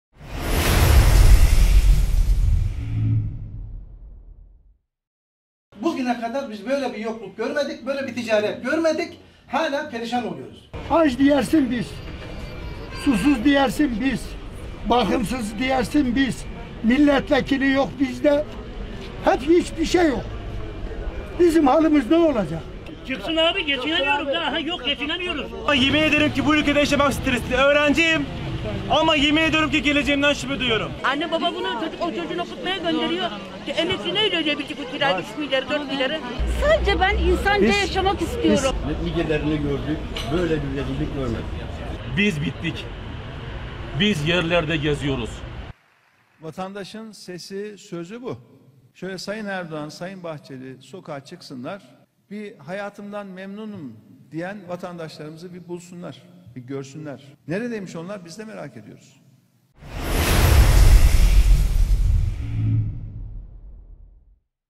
Ali Babacan sordu: Vatandaş açtı ağzını yumdu gözünü
Deva Partisi lideri Ali Babacan Türkiye gezilerine devam ediyor. Ali Babacan vatandaşlara dertlerini sordu, vatandaşlar açtı ağzını yumdu gözünü.